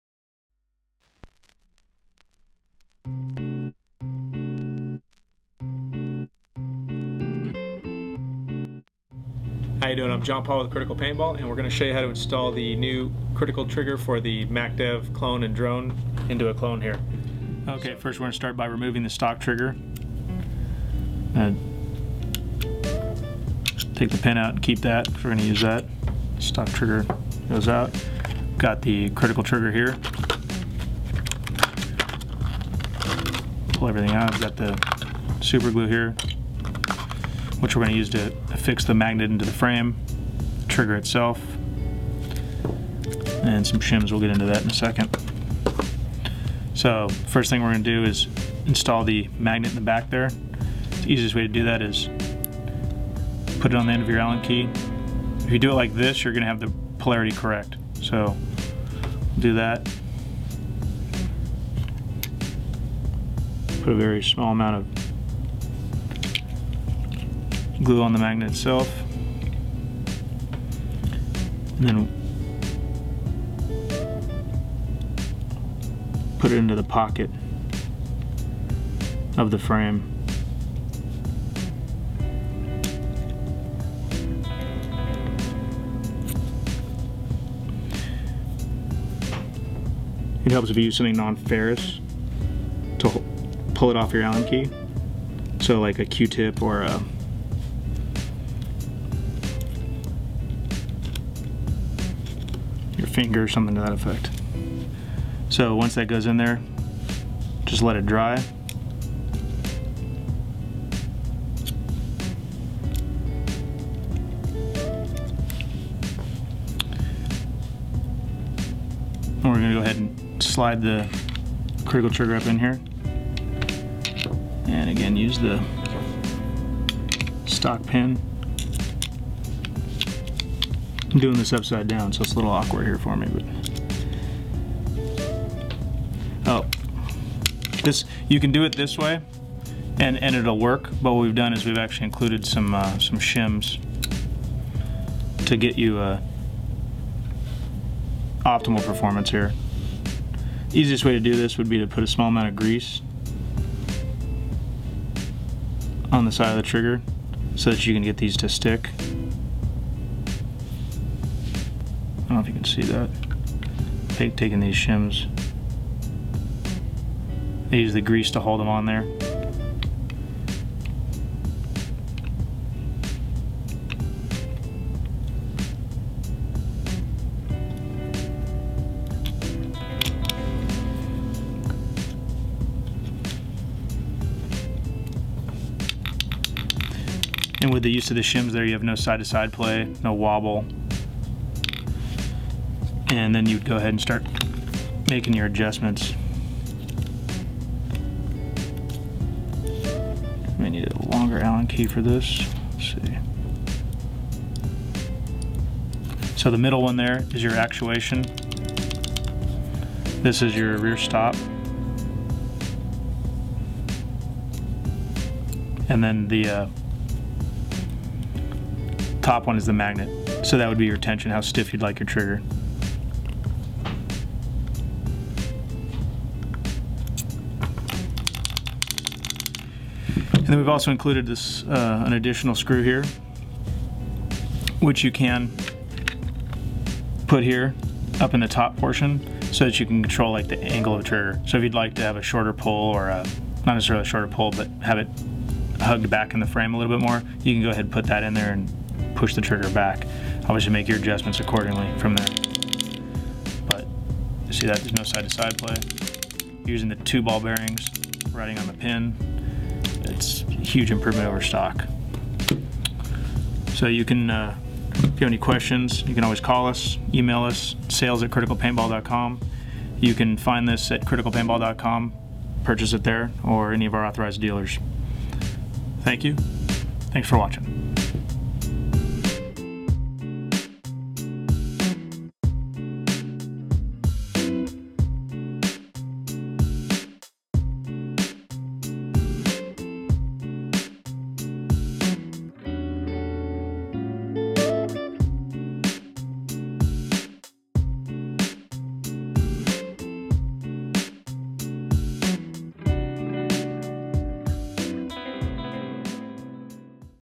Critical Trigger for the MacDev Clone / Drone
Install instruction for the Critical Paintball MacDev Clone / Drone Trigger

The Critical Paintball MacDev Clone / Drone Trigger will fit the Clone VX, Clone GT, Drone DX, and Tactical Drone.
    - Dual Ball Bearing Pivot Point (Ultra Smooth High Quality Bearings)
    - Ultra Light Weight Design and Ergonomically Correct Design
    - Adjustable Reverse Polarity Magnetic Return
    - 4 way adjustable design (switch activation, trigger angle, rear stop,
      and magnet tension)
    - Rounded thicker design and Smooth Edges makes this trigger
      feel more comfortable than stock
    - Super Smooth Trigger Pull and Very Fast
    - Available in Black Dust, and Black Polished

film & edited by Randy Felix